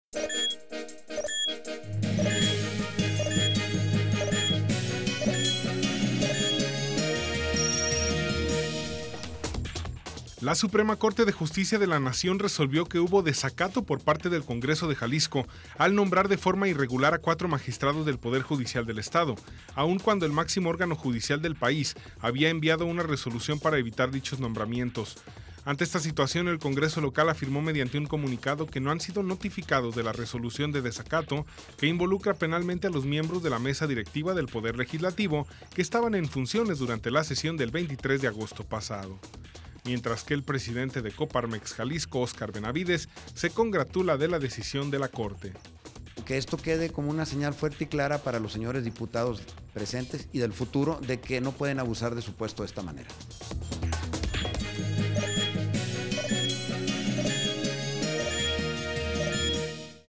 Corte 44 - Hubo desacato por el Congreso de Jalisco 12 de Abril de 2012
La Suprema Corte de Justicia de la Nación, resolvió que hubo desacato por parte del Congreso de Jalisco, al nombrar de forma irregular a cuatro magistrados del Poder Judicial del Estado, aun cuando el máximo órgano judicial del país,  había enviado una resolución,  para evitar dichos nombramientos.

Ante esta situación,  el Congreso Local, afirmó mediante un comunicado que no han sido notificados de la resolución de desacato, que involucra penalmente a los miembros de la mesa directiva del Poder Legislativo,  que estaban en funciones durante la sesión del 23 de agosto pasado.

Mientras que el Presidente de COPARMEX Jalisco, Óscar Benavides,  se congratula de la decisión de la corte.